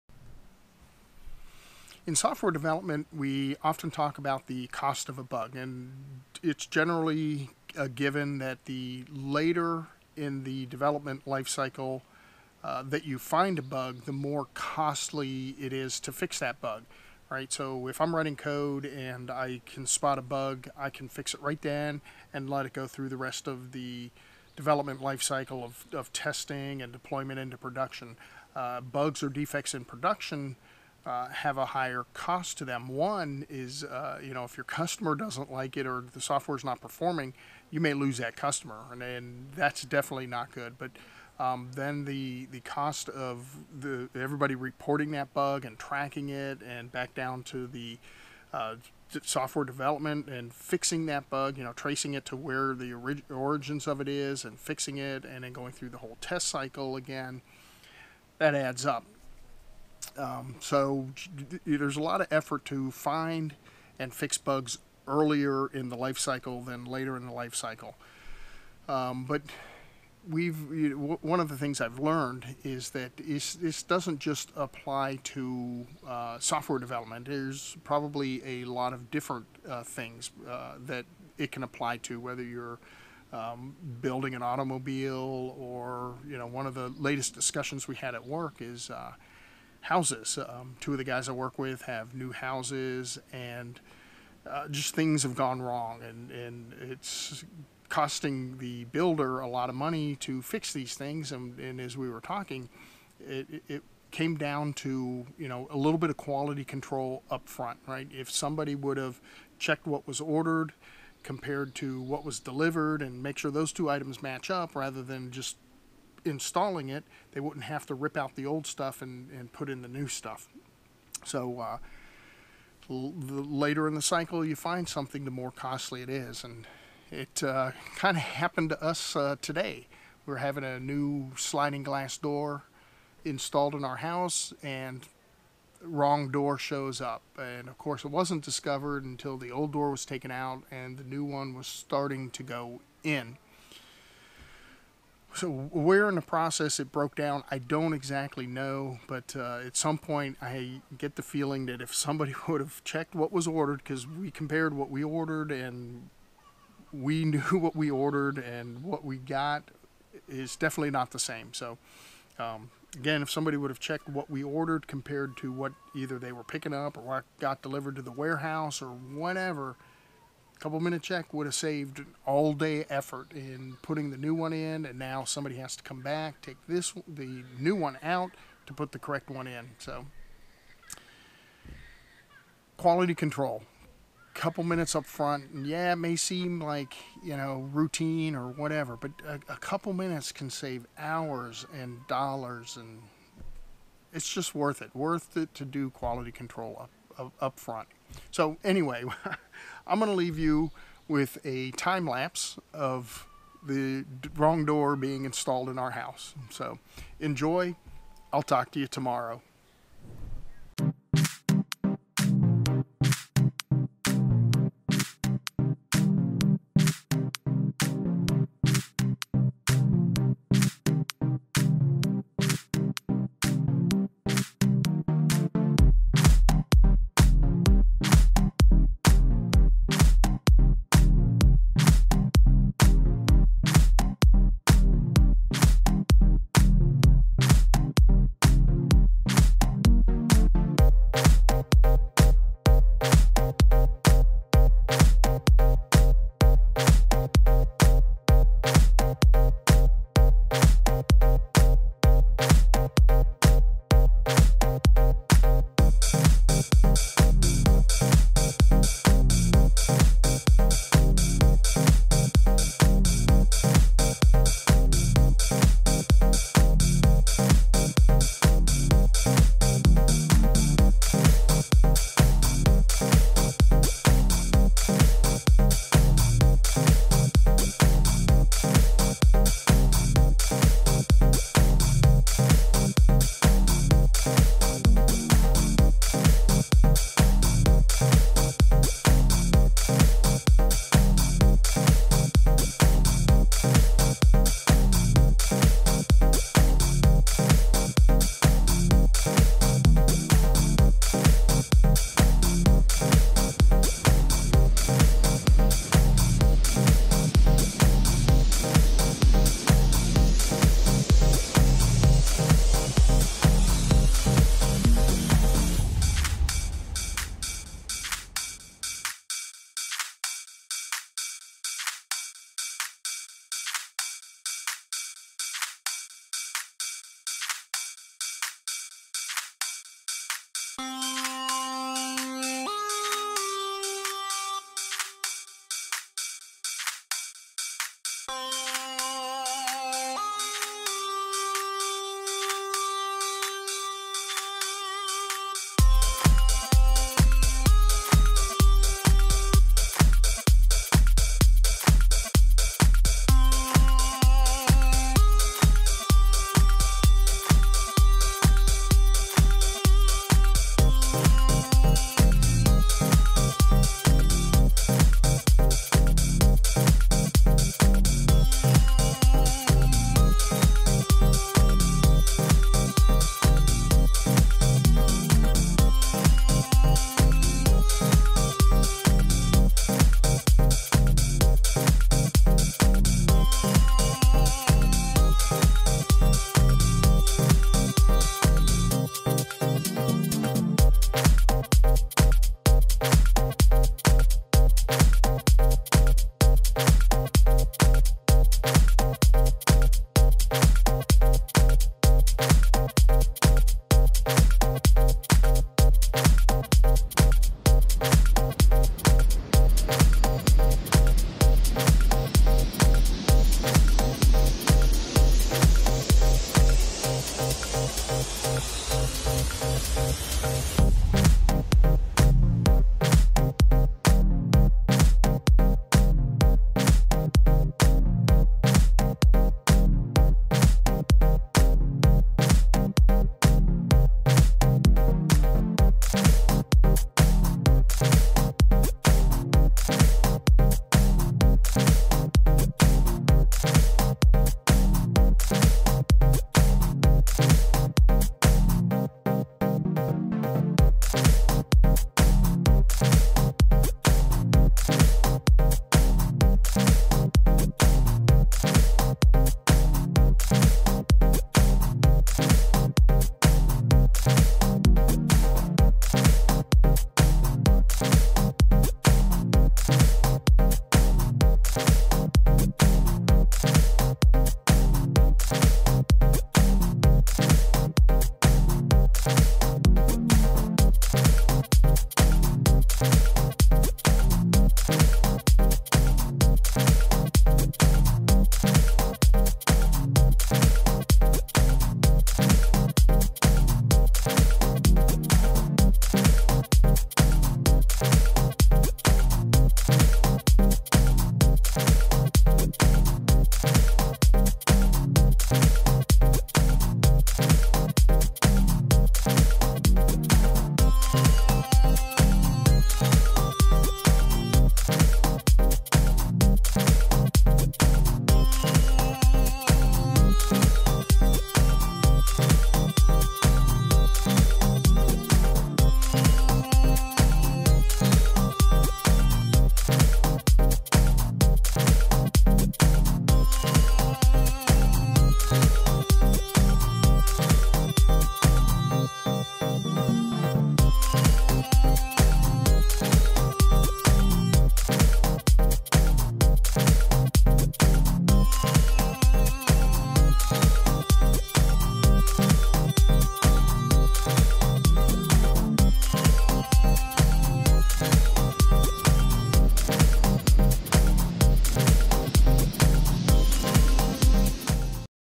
Home Project Gone Wrong!
Quality control up front can save a lot of time and cost later! My thoughts on why quality control earlier in the process is so important!